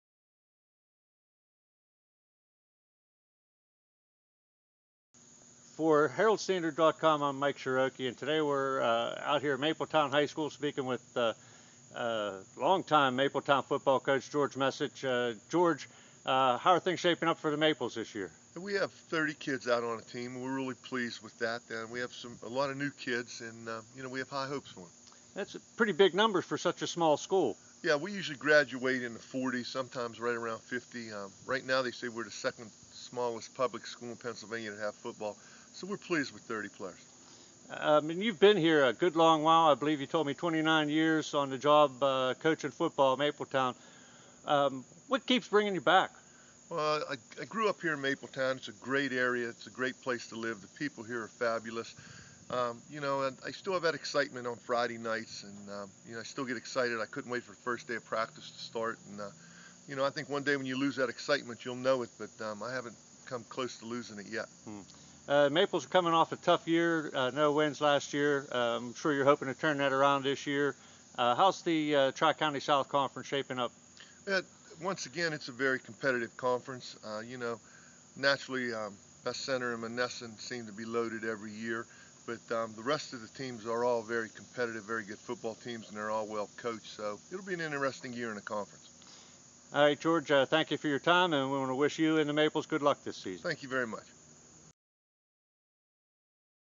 Mapletown head coach George Messich
Coach George Messich talks about the season ahead facing Mapletown